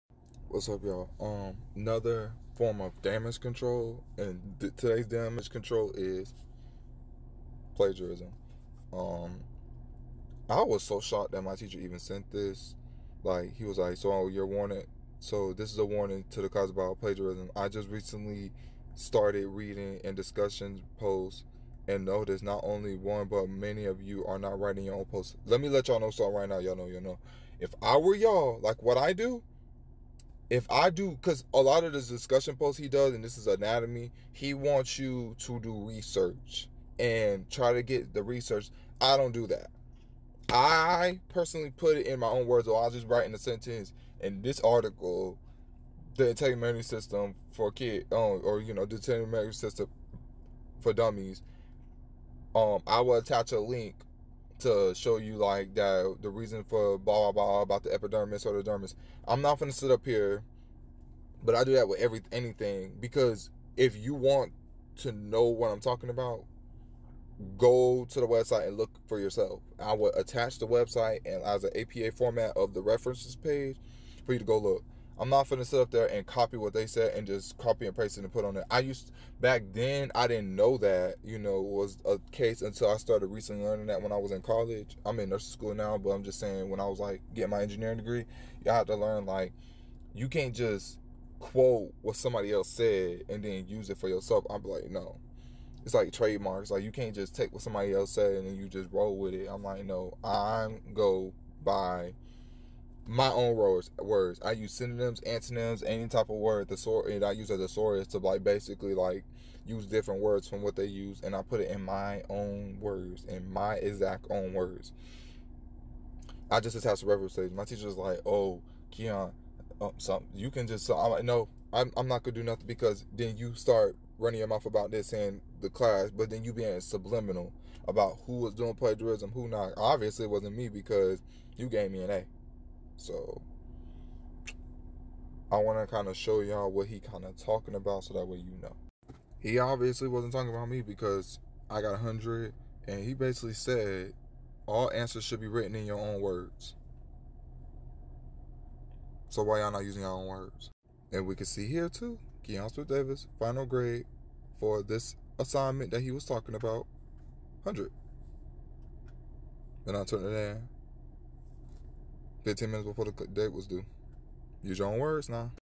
Nursing school Teacher Caught Someone Cheating! [Plagiarism]
Remember: Please do your own work. I know school is stressful, but it's better than getting kicked out.